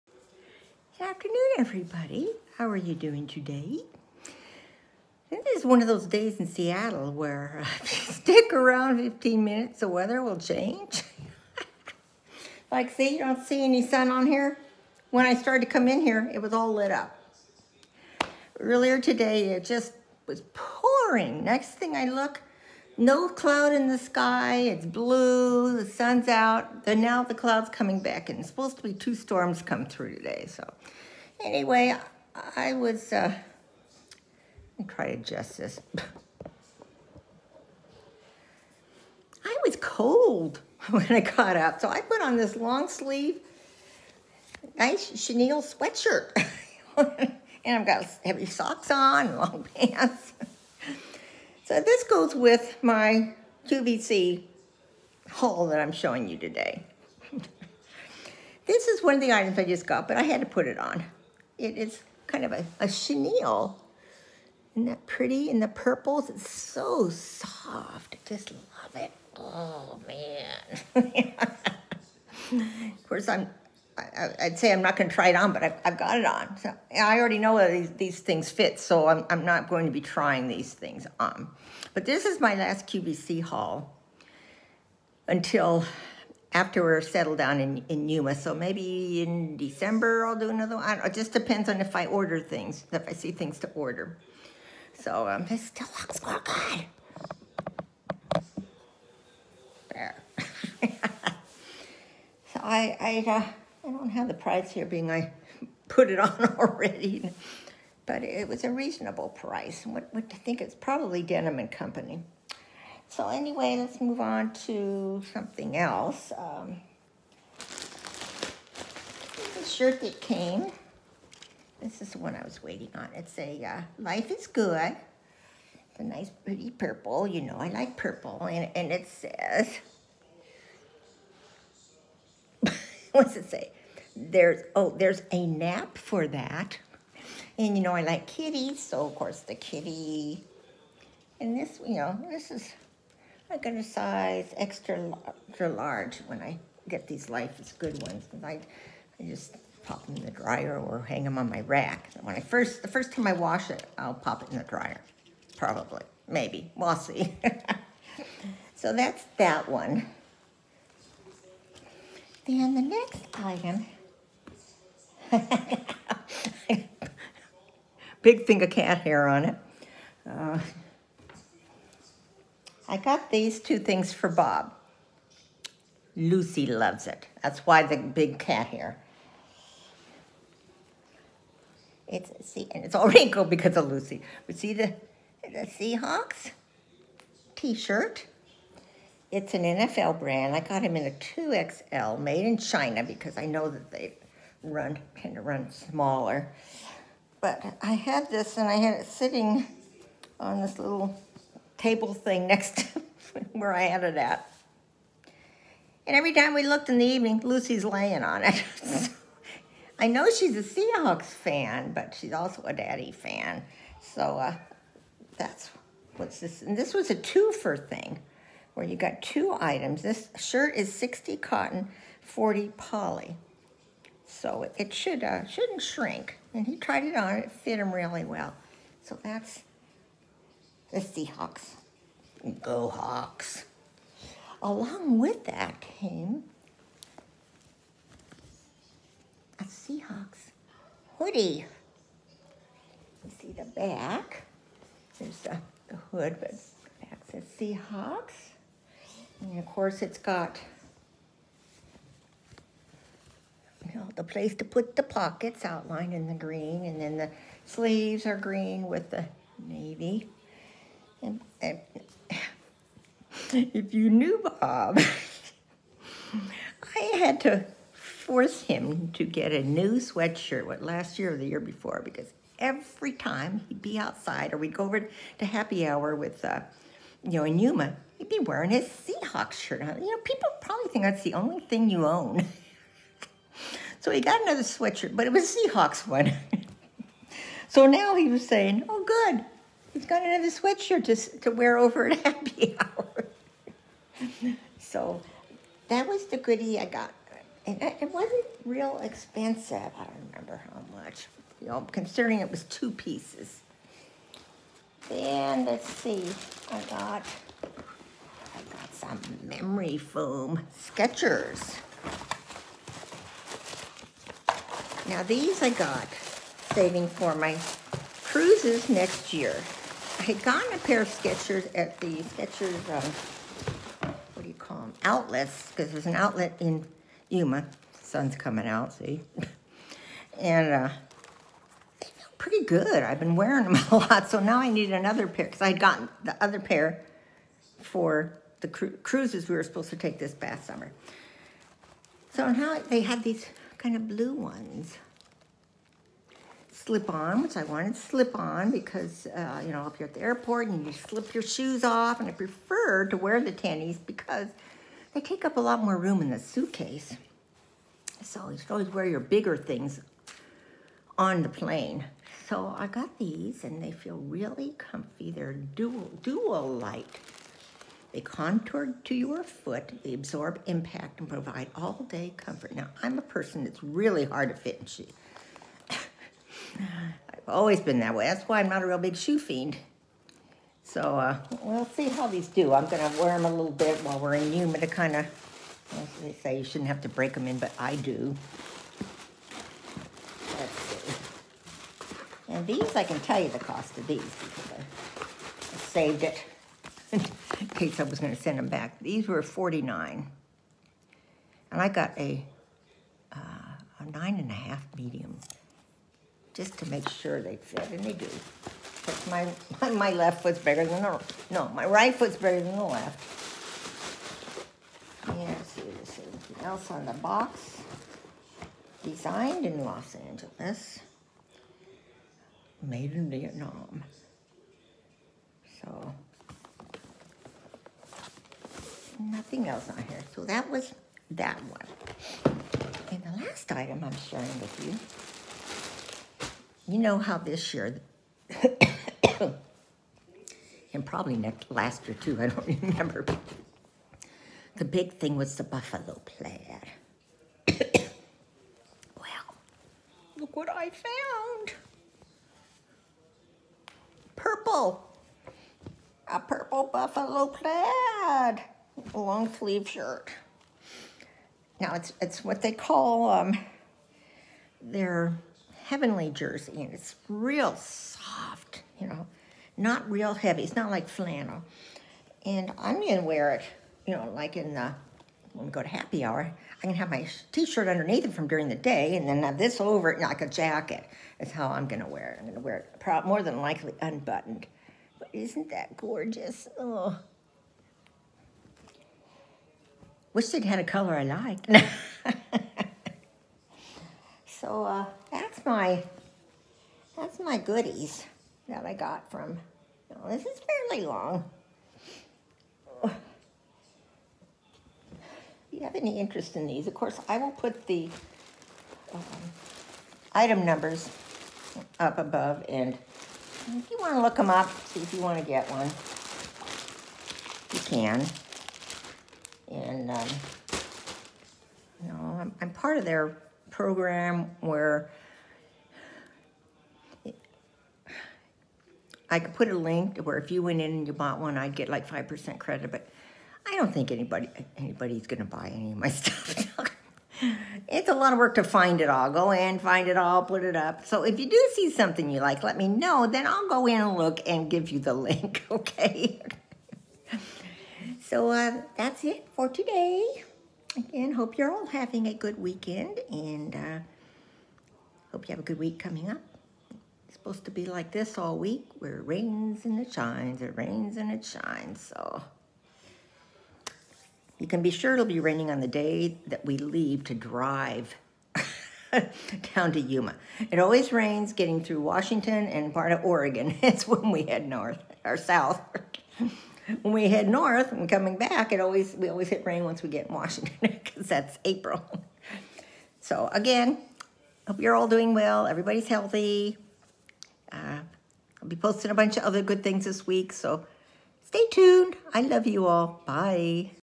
QVC Clothing Haul
Join me as I share items I purchased from QVC in the last few months.